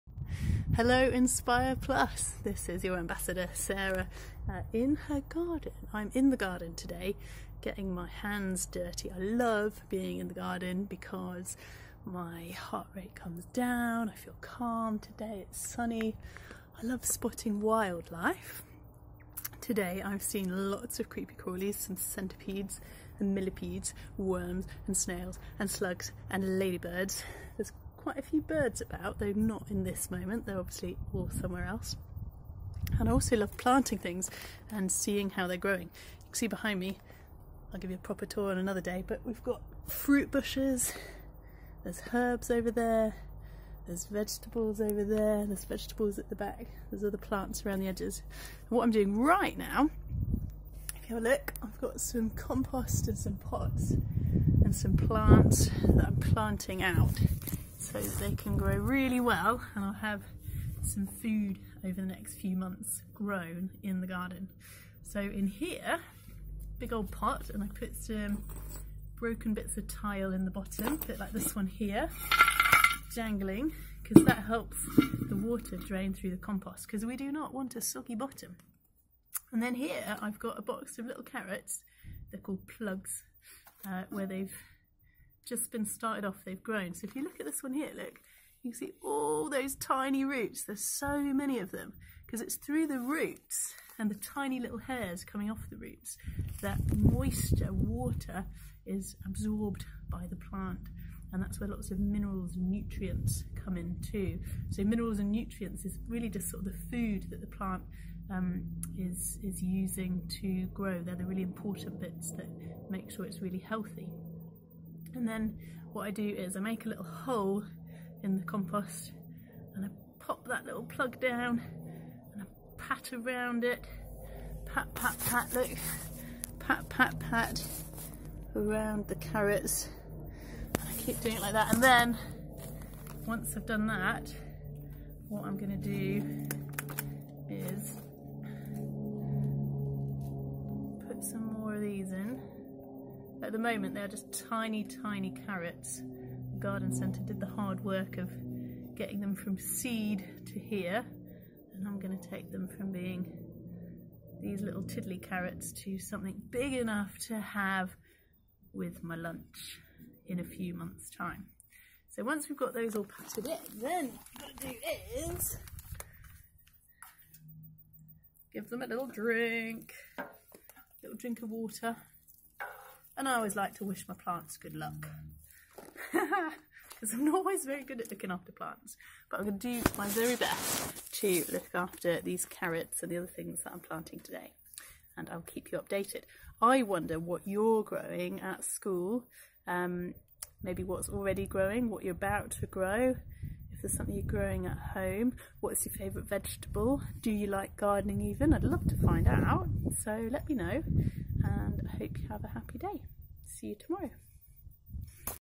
Sarah Outen- Daily vlog 2
Our ambassador, Sarah Outen is busy in her garden today planting some carrots!🥕 Join Sarah as she explains how to plant them!🌱

Have you been doing some gardening today? Let us know by commenting below!👇🏻